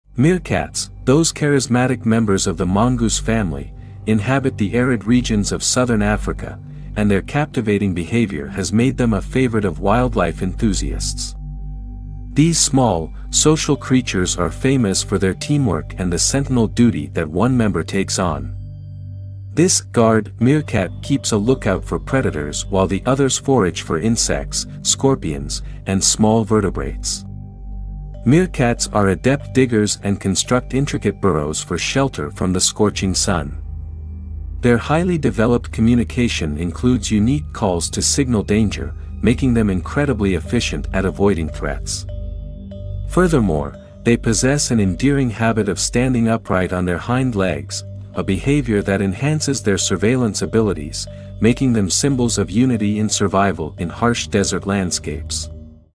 Meerkat Spotlight: Exploring Africa's Guardians
Explore the intriguing lives of meerkats in their desert home. Join us for captivating wildlife encounters, heartwarming moments, and in-depth insights into these charming desert dwellers.